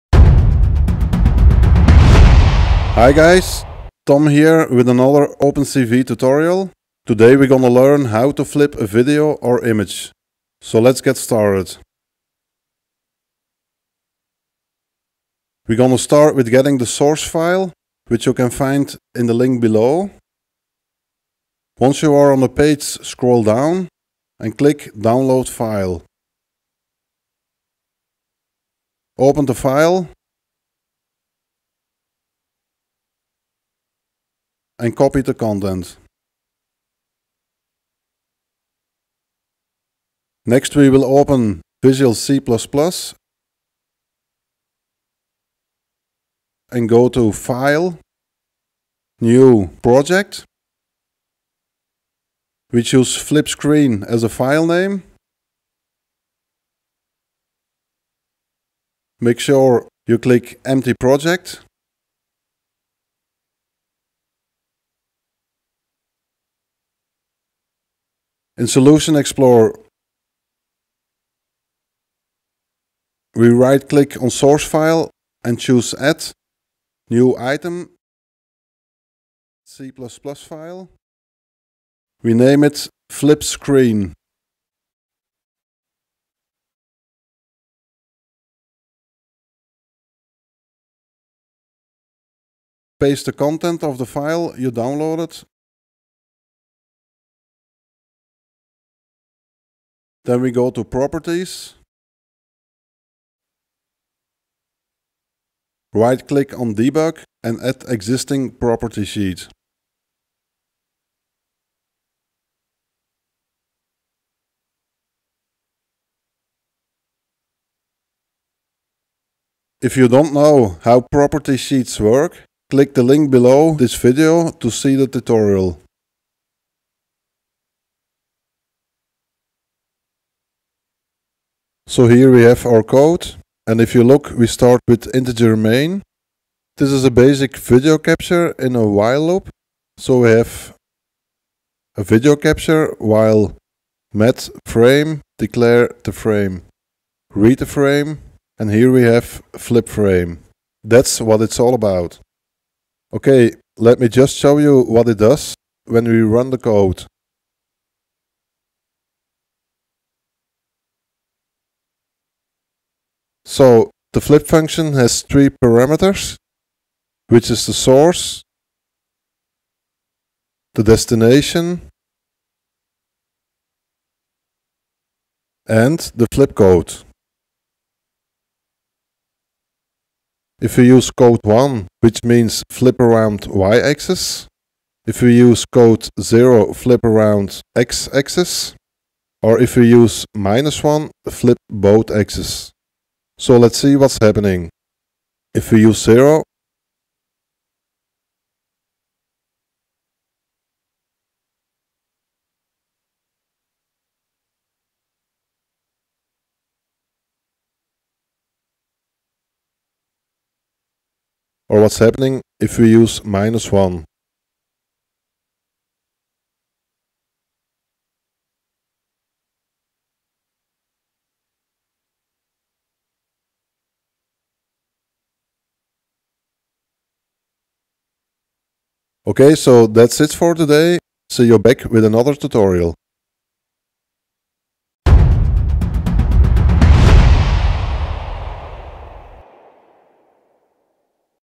Flip image or video using openCv
Flip or rotate an image or video using openCv on X-axis or Y-axis.